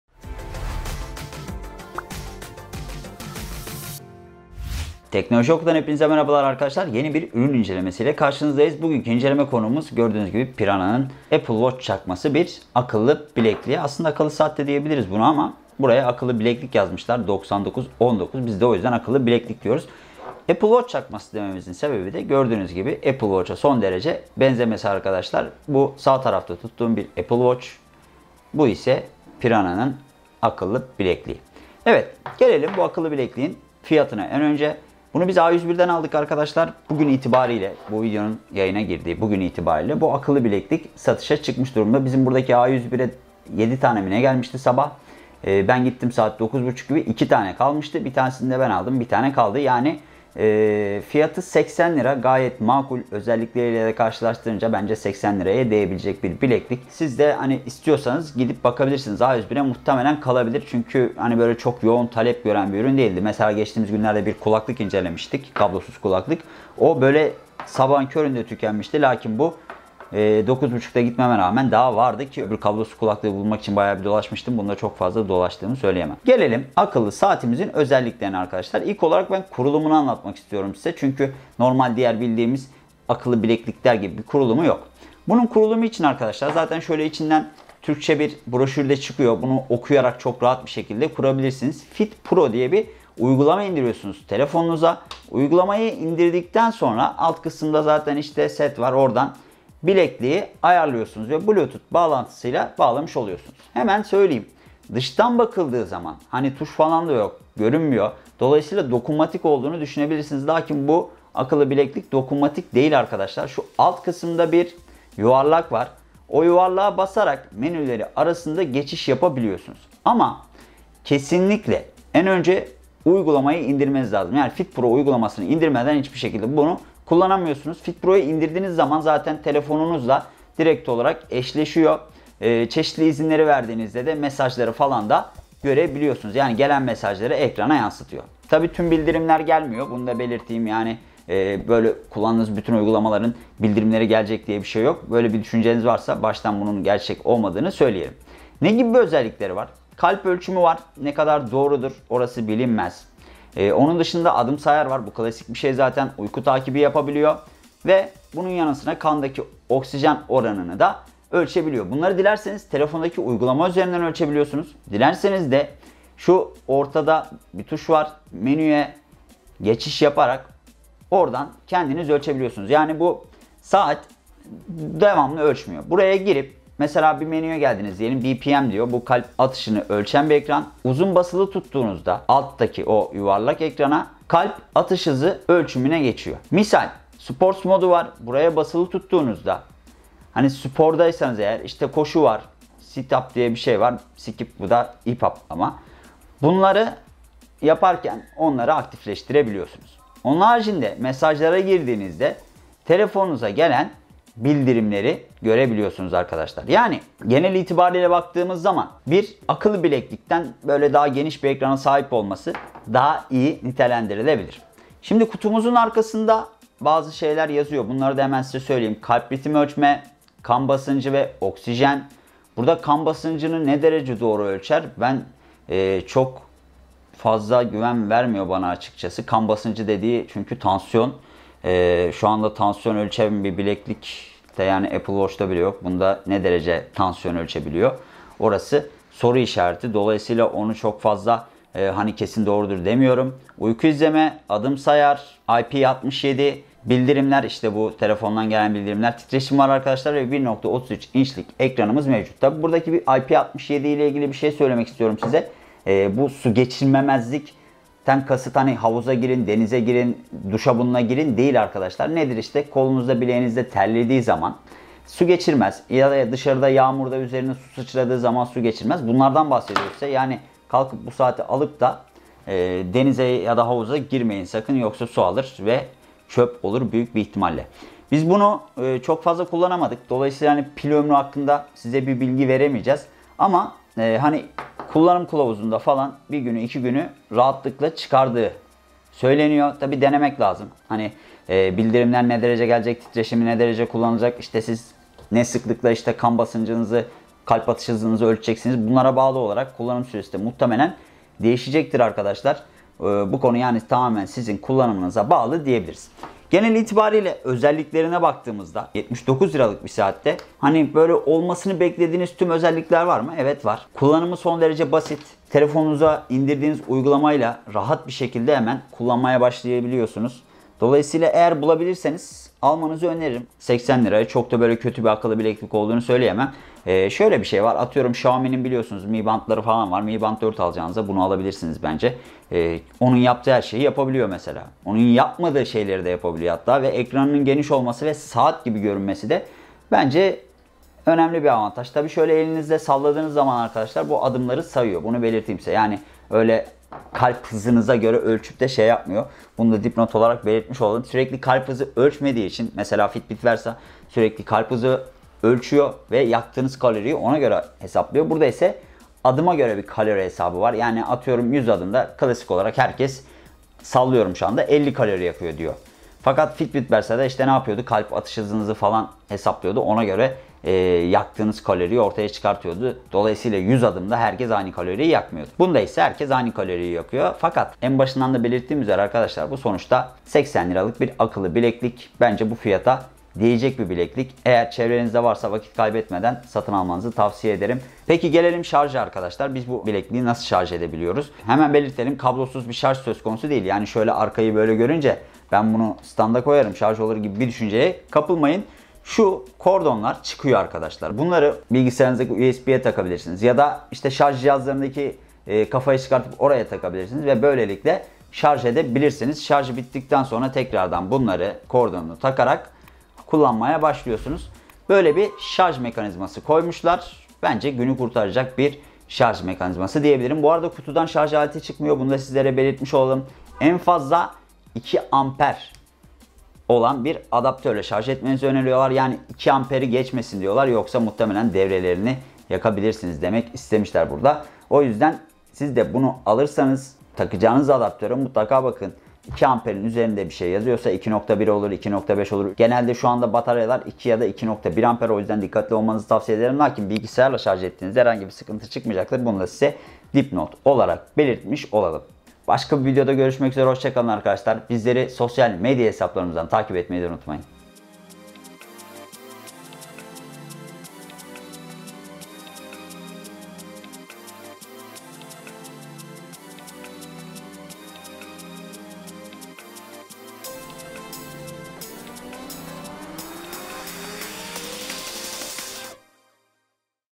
A101'de satılan çakma Apple Watch! Piranha akıllı bileklik!
Son dönemde akıllı bilekliklerin hayatımızdaki etkinliği önemli ölçüde arttı. Elbette bunda akıllı bileklik ve akıllı saatlerin fiyatının makul seviyelere düşmesi de önemli rol oynadı.

Hali hazırda 200 TL altına oldukça kaliteli bileklikler almak mümkün. Eğer bütçeniz biraz daha fazlaysa o zaman seçenekler de artıyor.

Lakin 100TL altında bir bütçeye sahipseniz alabileceğiniz akıllı bilekliklerin sayısı da hayli kısıtlanıyor. Fakat A101'de satılan Apple Watch benzeri akıllı bilekliğin tam da size göre olduğunu söyleyebiliriz.

80TL'lik çakma Apple Watch! Bu fiyata en iyisi!

Piranha tarafından üretilen bu akıllı bilekliğin oldukça fonksiyonel olduğunu söylemek mümkün. Dilerseniz ürünün incelemesine aşağıdan göz atabilirsiniz.